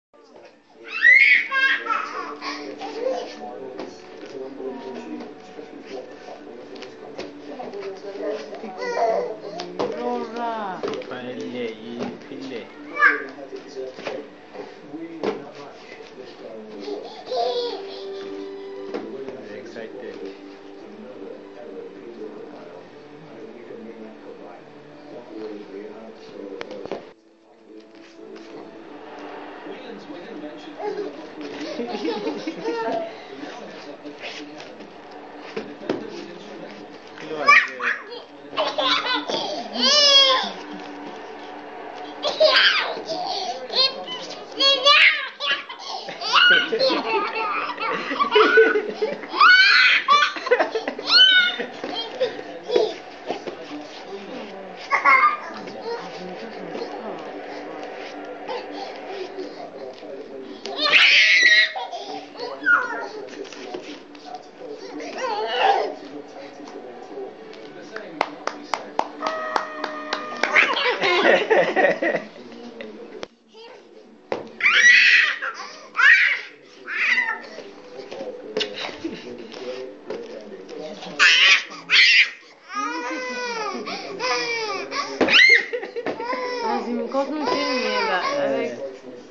Never give up...never ever....
King Olulu's son learning to walk. This is one of life's key lesson, you did not give up trying to walk, why do you want to give up on your dreams? When you fall, get back up and try again and again until you win...